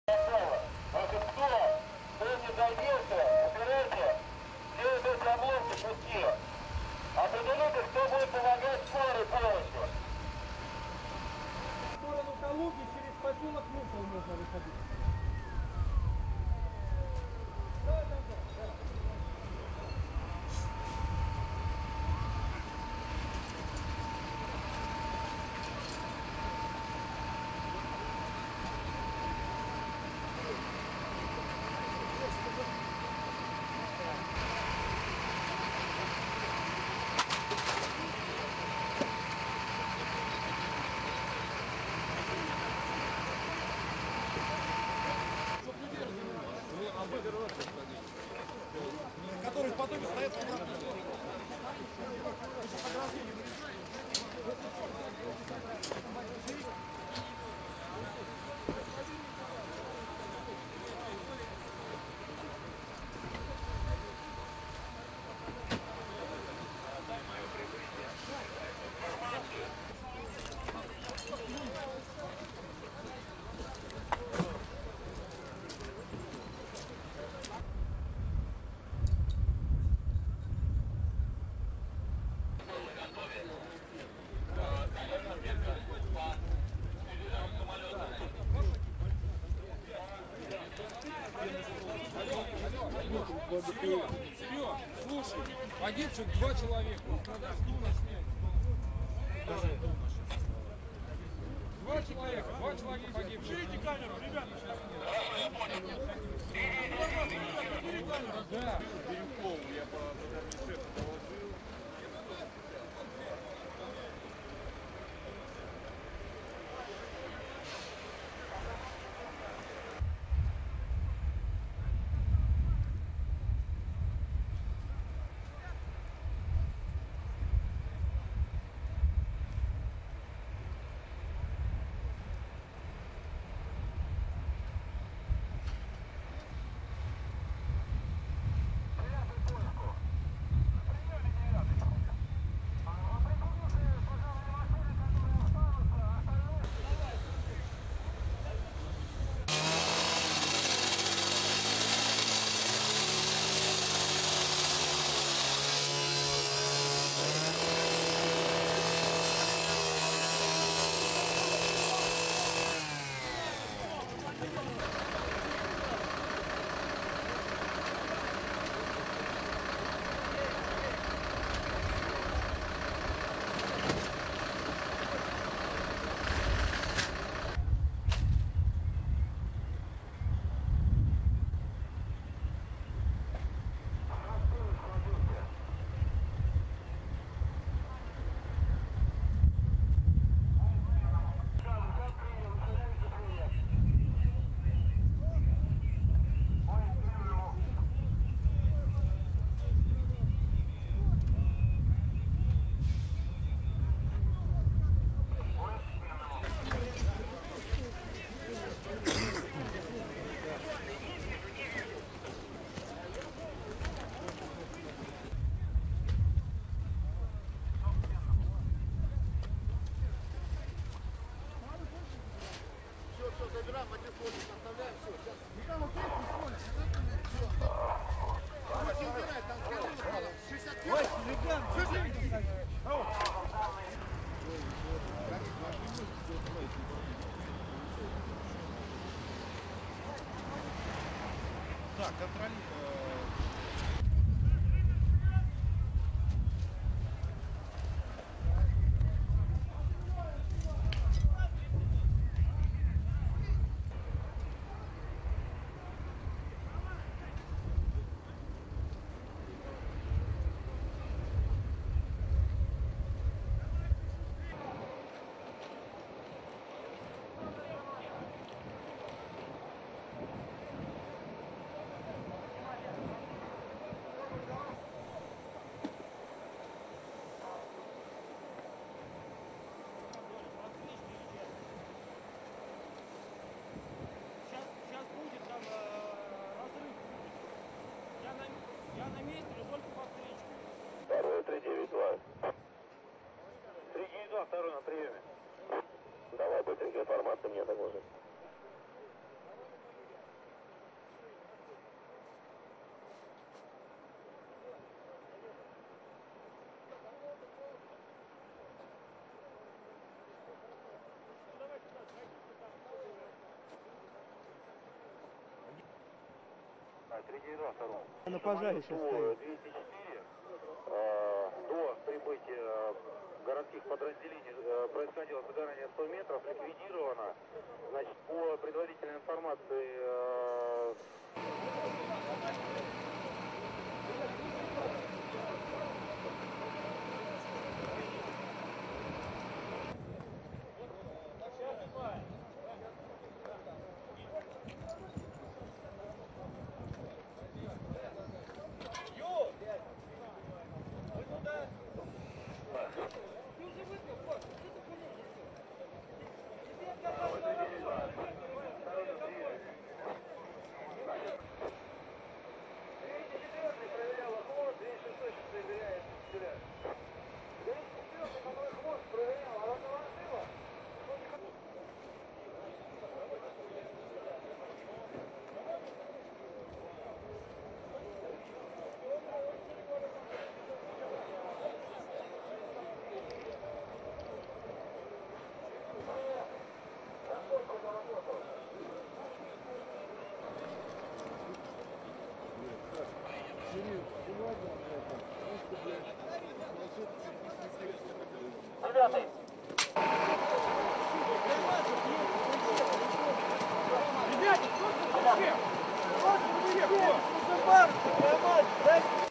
Clear footage of RUSSIAN CRASH site on motorway.mp4
Story
A passenger plane crash landed on the Kievskoye highway outside Moscow's Vnukovo Airport on Saturday, after overshooting the runway.

According to the Russian Interior Ministry, four of the eight crewmembers on board died. No passengers were on the plane.

The TU-204 airliner owned by Red Wings was returning from Pardubice in the Czech Republic.

A total of 124 rescue workers and 12 vehicles from the Russian Emergency Ministry were dispatched to the crash site.